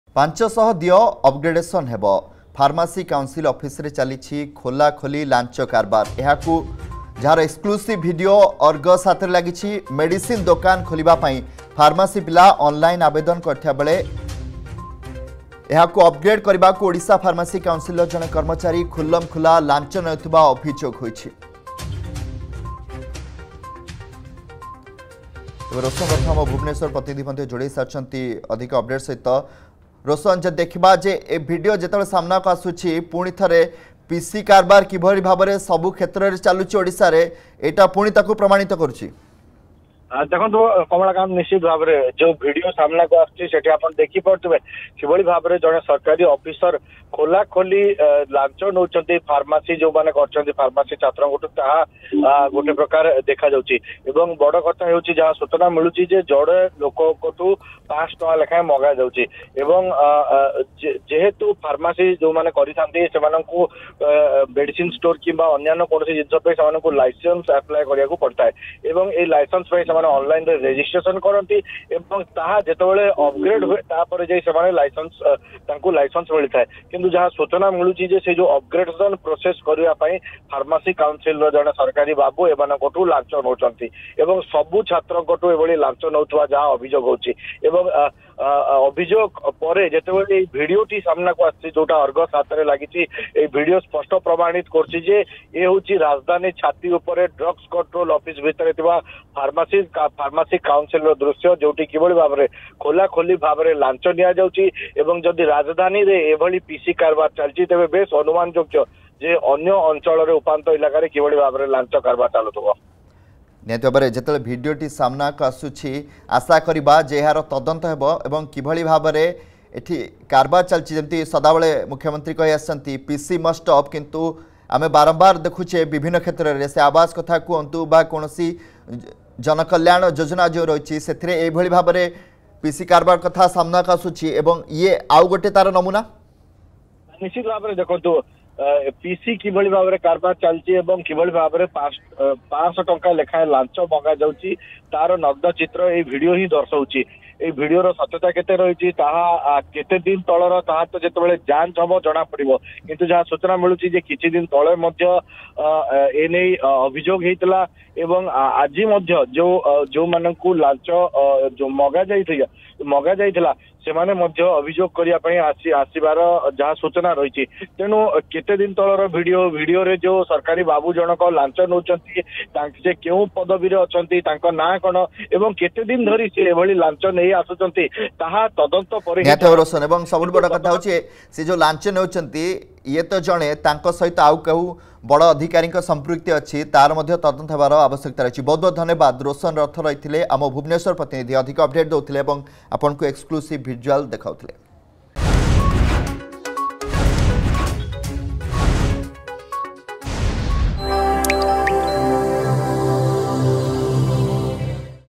Open Bribery in Pharmacy Council Office, Exclusive Video Clip with Argus News
Open Bribery in Pharmacy Council Office, Exclusive Video Clip with Argus News in Bhubaneswar.